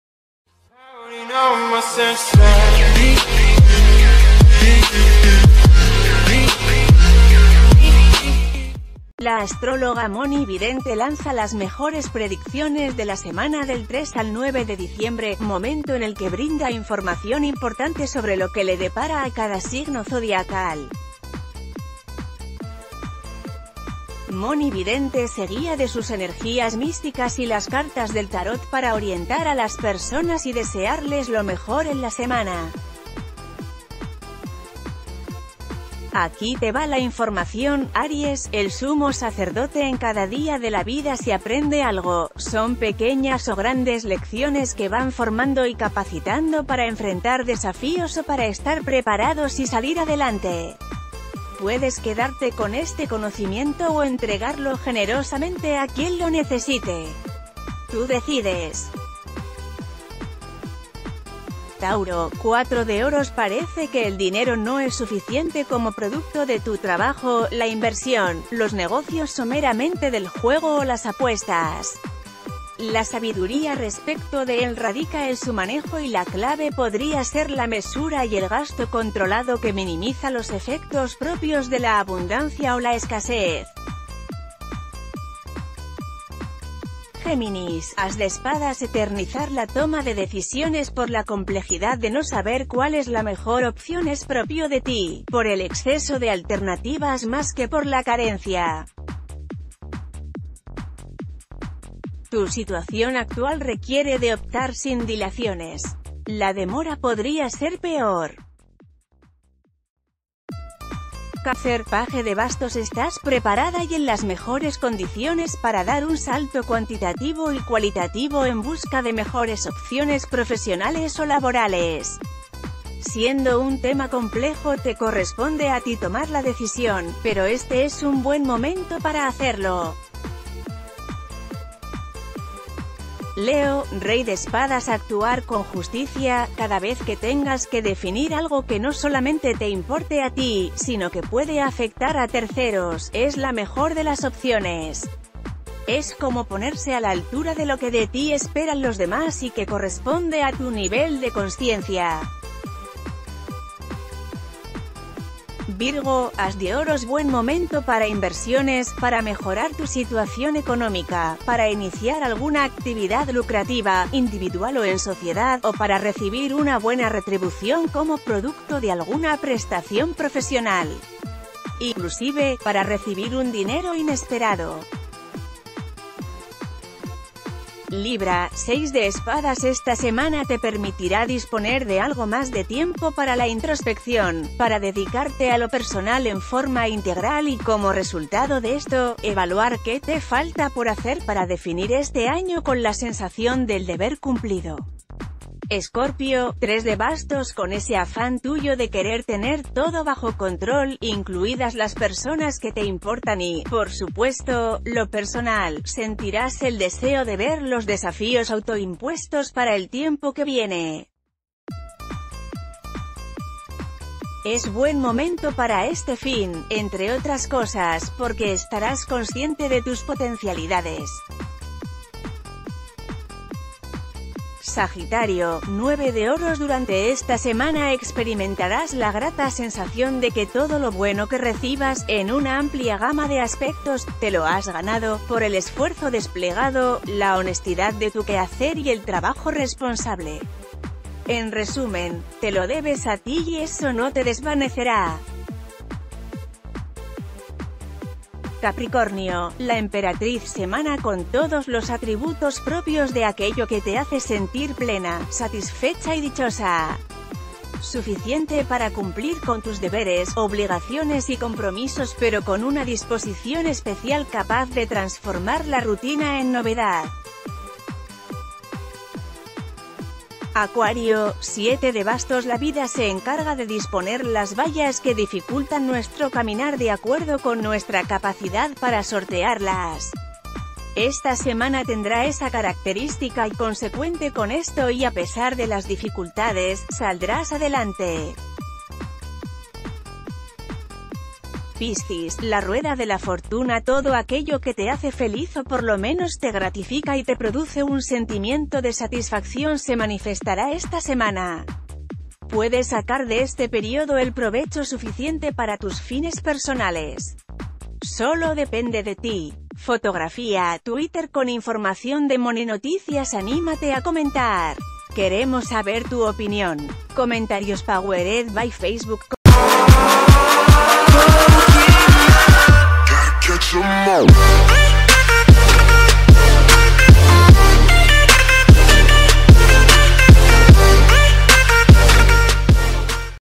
Mhoni Vidente revela qué carta del tarot traerá suerte a tu signo estos días
La astróloga Mhoni Vidente lanza las mejores predicciones de la semana del 3 al 9 de diciembre, momento en el que brinda información importante sobre lo que le depara a cada signo zodiacal. Mhoni Vidente se guía de sus energías místicas y las cartas del tarot para orientar a las personas y desearles...